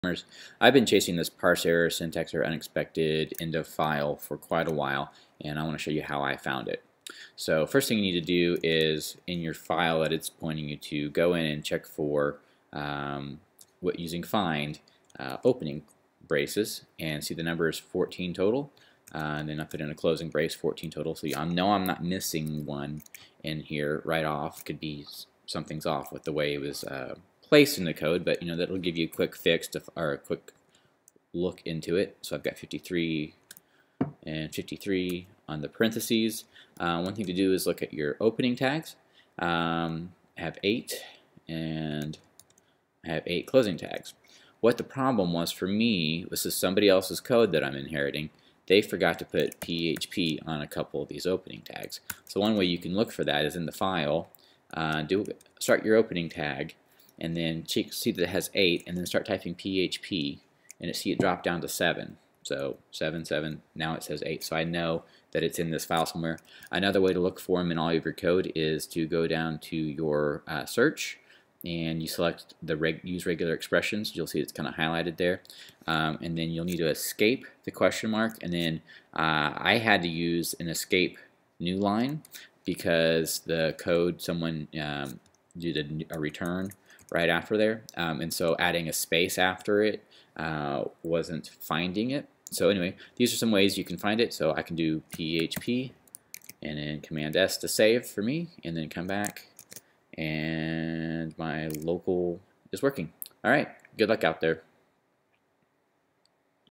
PHP HOW TO FIND: "Parse error: syntax error, unexpected end of file in..."
A quick guide on how to find the cause of PHP error "Parse error: syntax error, unexpected end of file in..." or "Parse error: syntax error, unexpected '}', expecting end of file in..."